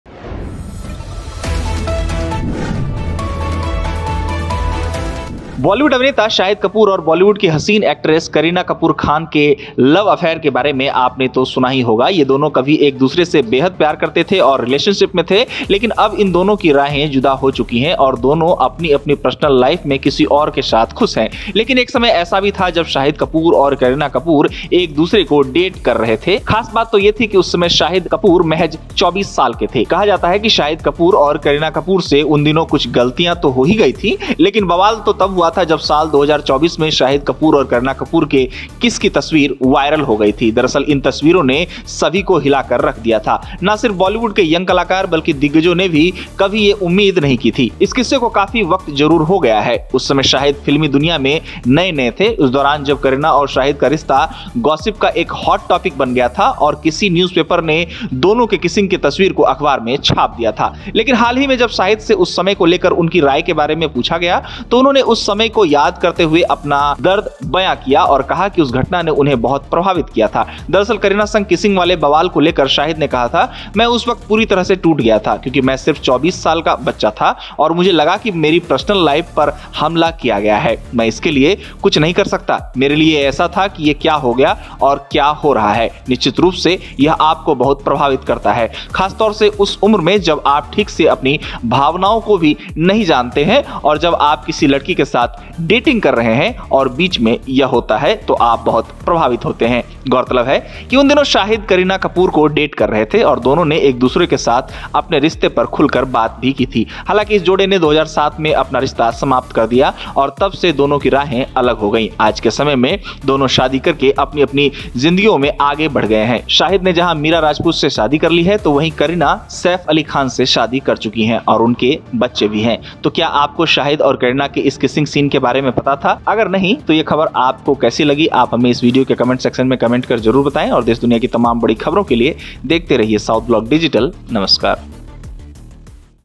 जब अख़बार में छप गई थी Karina और Sahid के Liplock की तस्वीर
बॉलीवुड अभिनेता शाहिद कपूर बॉलीवुड की हसीन एक्ट्रेस करीना कपूर के लव लाइफ के किस्से तो बॉलीवुड के गलियारों में आम हैं। आज भले ही दोनों की राहें जुदा हो चुकी हैं और दोनों अपनी –अपनी पर्सनल लाइफ में किसी और के साथ खुश हो, लेकिन एक समय ऐसा भी था जब शाहिद कपूर और करीना एक-दूसरे को डेट करते थे। खास बात तो यह थी कि उस समय शाहिद महज 24 साल के थे। उन दिनों जवानी के जोश में शाहीद और करीना से कुछ गलतियां तो हो ही गई थी, लेकिन बवाल तो तब हुआ था, जब साल 2004 में शाहिद कपूर और करीना कपूर के किस की तस्वीरें वायरल हो गई थीं। दरअसल, इन तस्वीरों ने सभी को हिला के रख दिया था। ना सिर्फ बॉलीवुड के यंग कलाकार बल्कि दिग्गजों ने भी कभी ये उम्मीद नहीं रखी थी।@southblockdigital